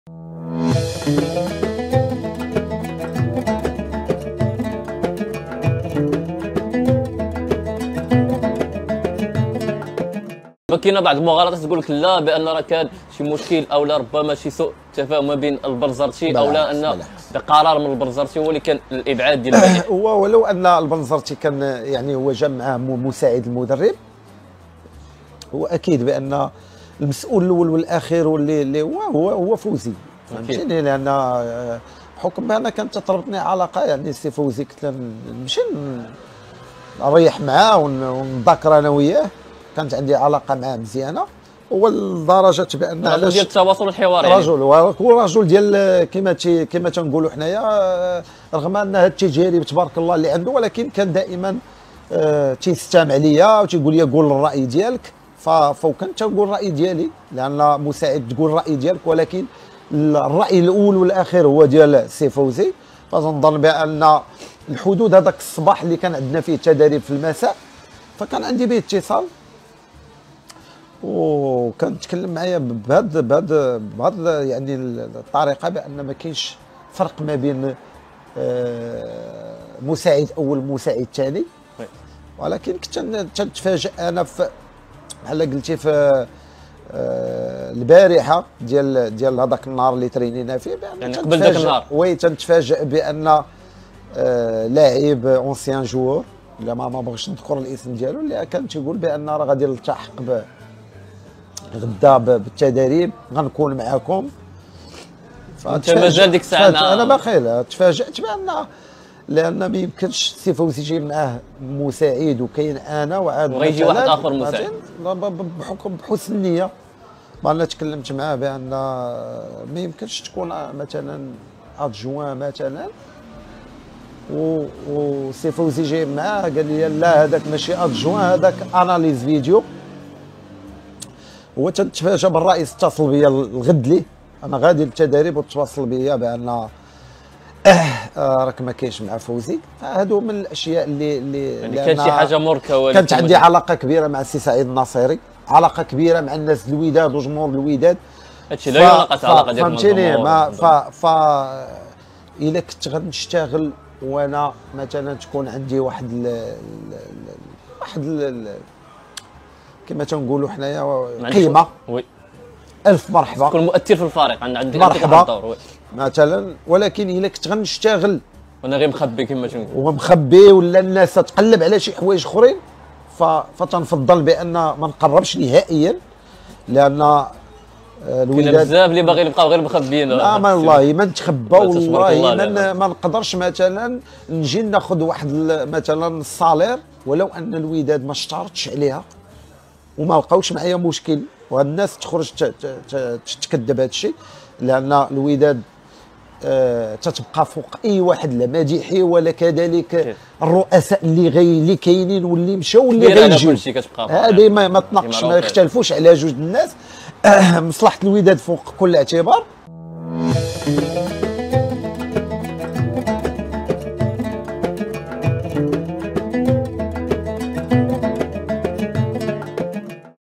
هام للجماهير الودادية.. ها شنو وقع لمديحي وكيفاش غادر أسوار الوداد الرياضي وحقيقة خلافه مع البنزرتي
كشف مديحي لشوف تيفي أسرار مثيرة وسبب رحيله عن الوداد وتفاصيل كثيرة..